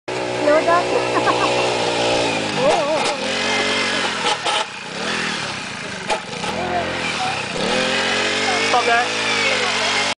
Racing at Farmosa, Malaysia (A)
Danial driving and Danish next to him.

Donna Lim, Singapore 2006